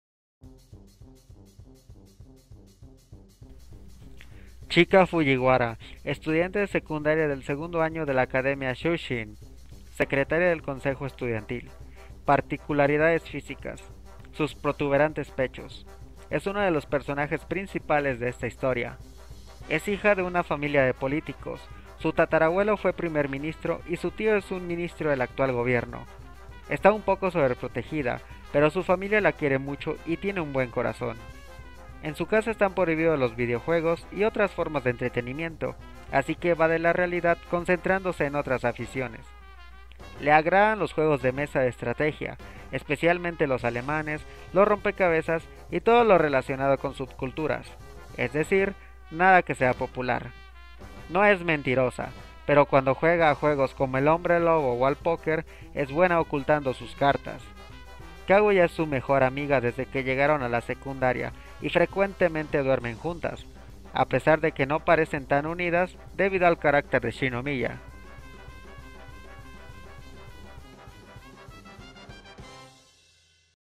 Quién es Chika Fujiwara? Descripción del Manga
Mi intento de narración tiene dificultades técnicas, un bajo audio y un zumbido en la grabación, pero igual se los dejo al igual que con KaguyaShinomiya, les dejo la descripción de ChikaFujiwara .